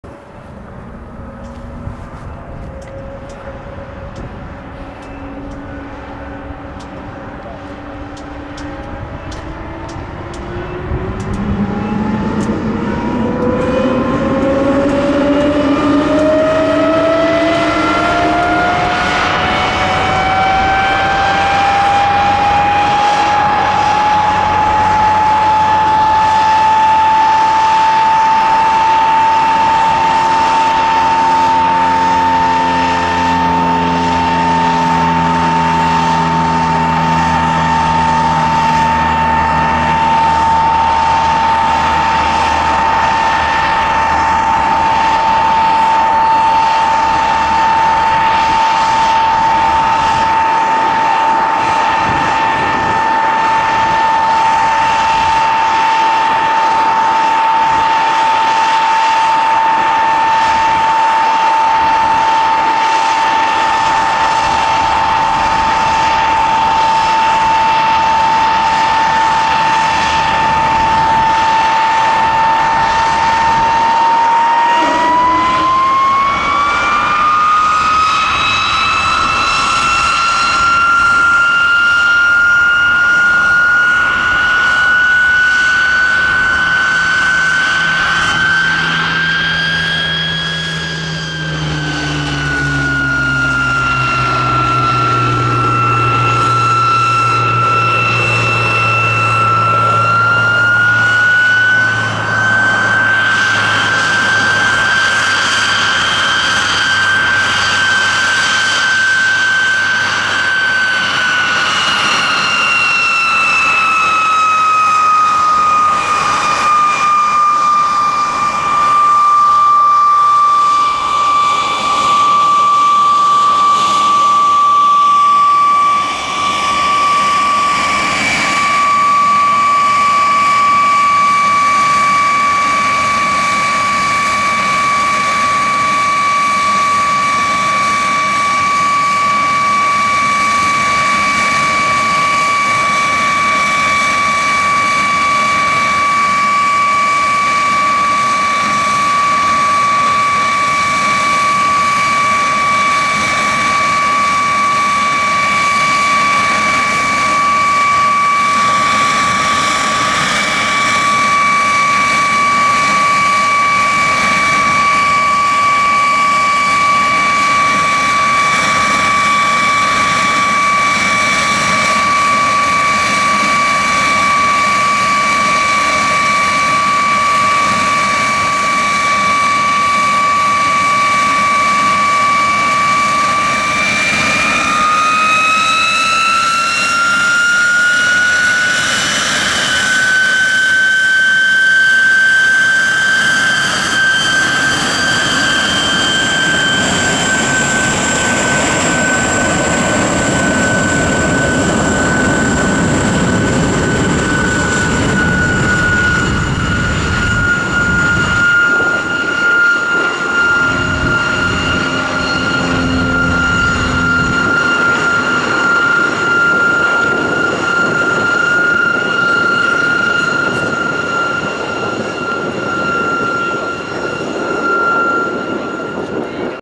[4k] De Havilland VAMPIRE Uncut Startup and Taxi + Bonus
De Havilland Vampire T.11 Uncut Start-up and taxi, with bonus Hawker Hurricane Takeoff in the sun at IWM Duxford!